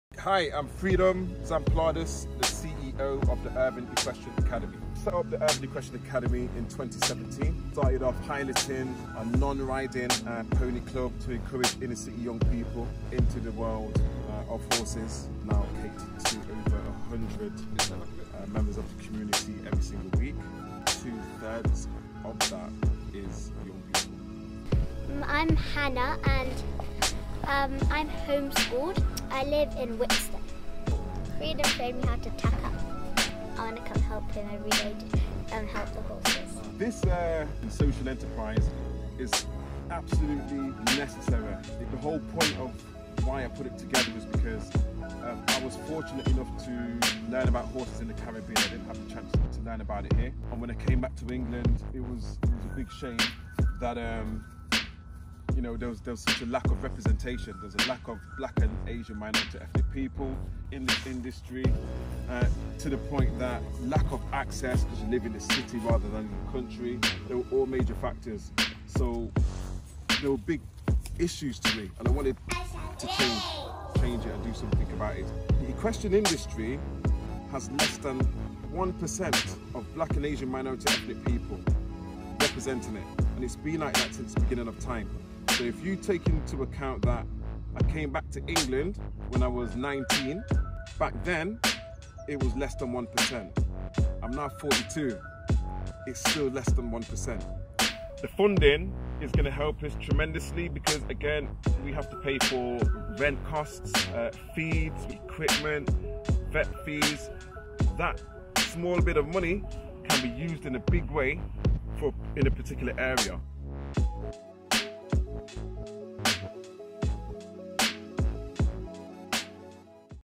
Urban Equestrian Academy: Sported case study
Hear from Urban Equestrian Academy who received a £2,000 donation from Simplyhealth through our partnership with Sported, thanks to your SimplyMe downloads.

Sported is a UK wide charity promoting fairness and equity for young people through grassroot sport and physical activity. They are the UK's largest network of community groups supporting 160,000 young people to overcome barriers to reach their full potential.

SimplyMe is our free health and wellbeing app that helps you stay track with your mental health, heart, sleep, weight and activity.